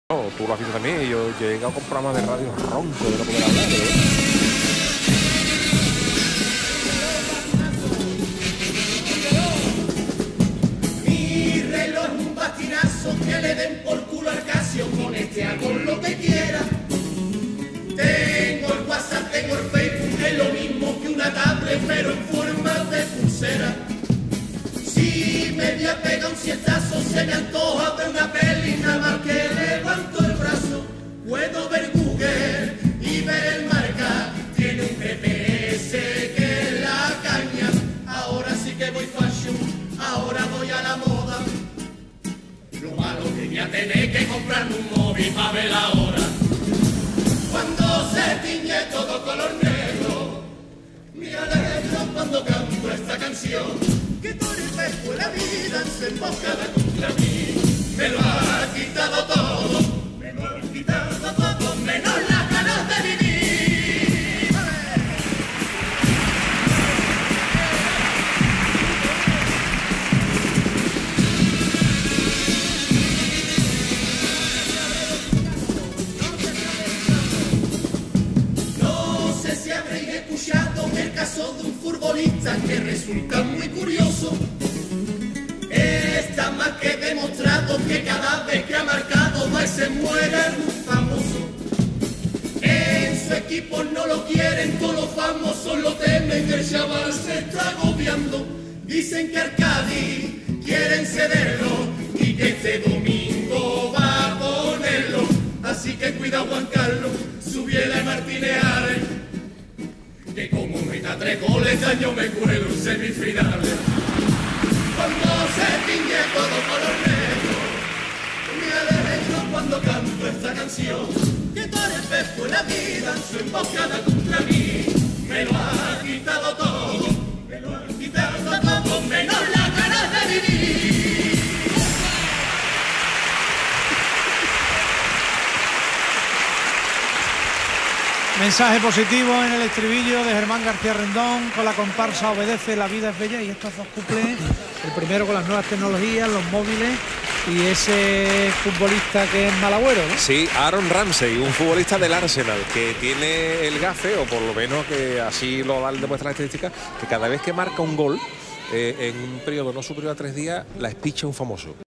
Comparsa OBDC. La vida es bella | Cuplés de CUARTOS
Sesión 1: Cuartos de Final del Carnaval de Cádiz 2016 | Emisión: 25/01/16

La comparsa del que fuera integrante de la chirigota de ‘El Canijo de Carmona’ y de la comparsa de Juan Carlos Aragón, Germán García Rendón, trae al Falla su quinta agrupación. Los cuplés de esta sesión de Cuartos son para los smartwatch, vamos los relojes ultramodernos que tanto se llevan ahora; y para el futbolista del Ársenal, Ramsey, que cada vez que marca un gol provoca la muerte de varios famosos.